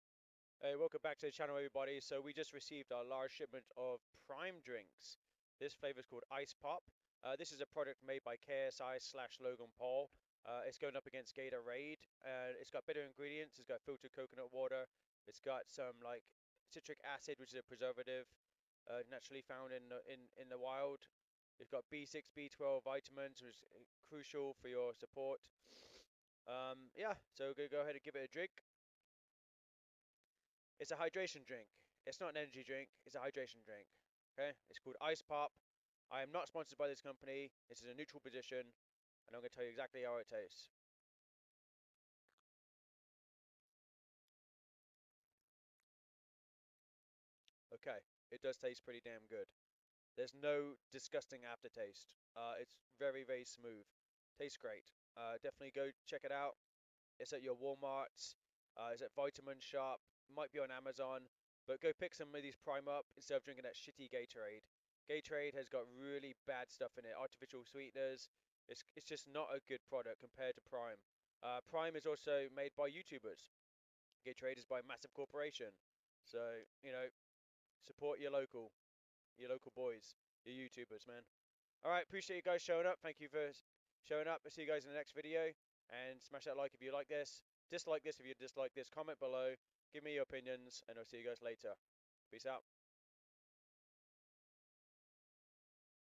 Opinion On Prime Drinks For Gaming Hydration
Today we received a shipment of Prime Hydration drinks